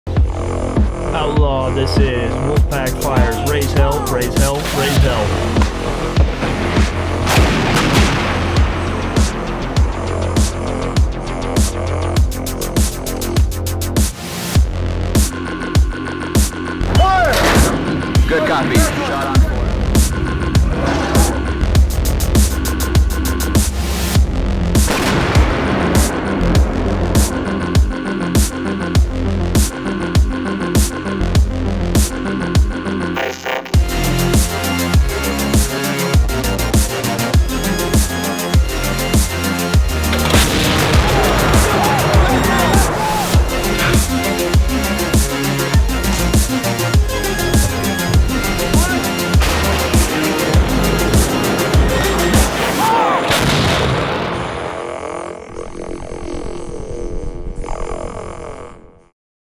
A strong Europe needs dynamic training
No nation can confront today’s challenges alone – Dynamic Front
demonstrates the U.S. resolve to stand side-by-side in Europe with our NATO Allies and partners.

Dynamic Front 18 will include approximately 3,700 participants from 26 nations,